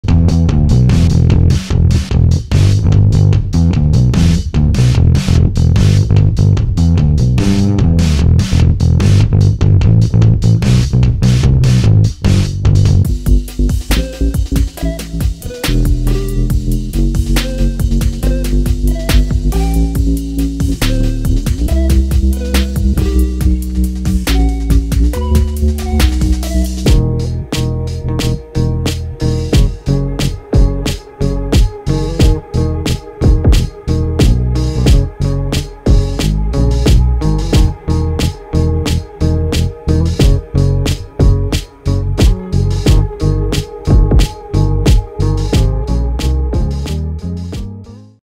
Tim Lefebvre Bass Volume 2
One of the most eclectic and inspiring bass collections ever produced is here. Introducing Tim Lefebvre (David Bowie, The Black Crowes) Volume 2, a new library featuring six unique sessions, each in its own distinct style, feel, key and tempo.

In addition, this collection features multitracks, allowing you to mix both of Tim's amped and direct bass tracks to your own taste. And to take things a step further, complimentary synth and atmospheric loops produced by Tim are also included. All of this is neatly organized into folders, allowing you to quickly drag and drop into the DAW of your choice.